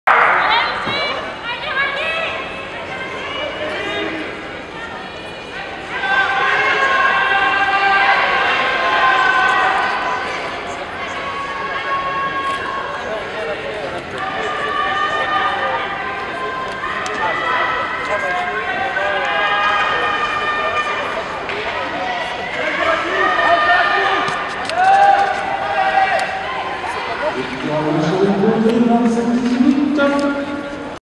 Championnat de Bruxelles Indoor - 200m Dames - Série 4
1 BAUDREZ CHARLINE - CABW - 27.17
2 AUCLAIR PAULINE - FRA - 27.46
3 DHEUR GéRALDINE - WS - 27.49
4 MORENCY-POILVACHE ALIZEE   -RESC - 27.78